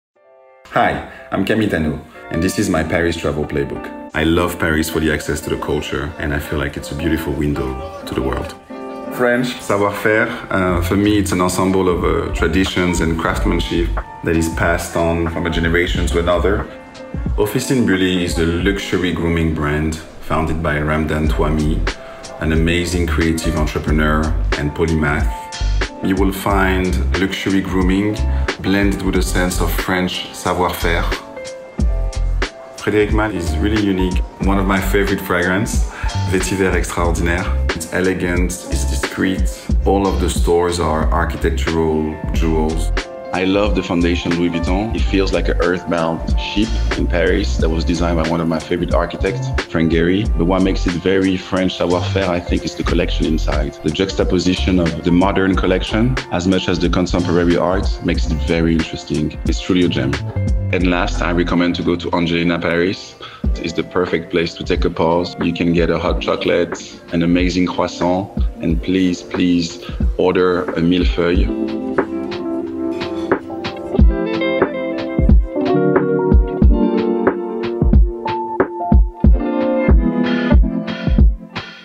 Atout France x EA
Experience the vibe of Paris with Camille Tanoh. A renowned fashion and product designer, Camille grew up immersed in the cultural fabric of the city, which is a major source of his creative inspiration.

We thank the Fondation Louis Vuitton, the vision of architect Frank Gehry, and note the copyright as retained by Gehry Partners, LLP and Frank O. Gehry.

CREDITS
Joan Mitchell - "La Grande Vallee XVI, Pour Iva”
Joan Mitchell - "La Grande Vallee IX”
Joan Mitchell - "La Grande Vallee”
Joan Mitchell - "La Grande Vallee XIV (For a Little While)”
Frank O. Gehry - "Sketch of the Foundation Louis Vuitton”
Olafur Eliasson - "Inside the horizon”
Katarina Grosse - "Canyon”